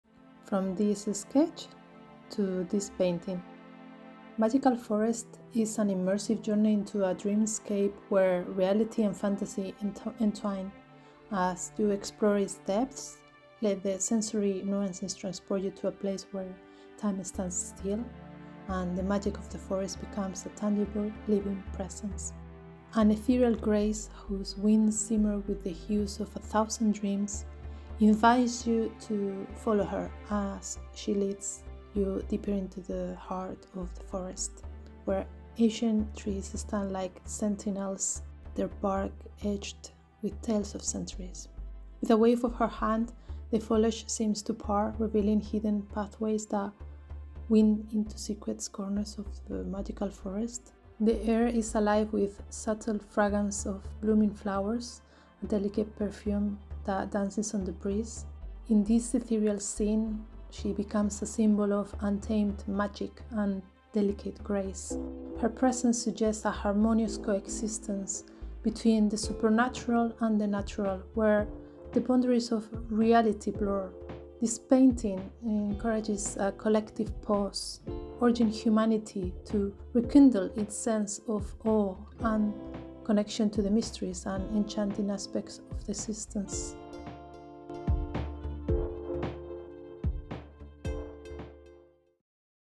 Ink Brush in Procreate ✿ Digital painting process
In this time-lapse video, I share my process when painting digitally in procreate. My line work is done with an ink brush from procreate after having clear the concept from my paper sketch. I also share the story and concepts behind this artwork.

I hope you enjoy the process, spark your creativity and learn how to paint with me :)

Raquel
Fine Artist & Illustrator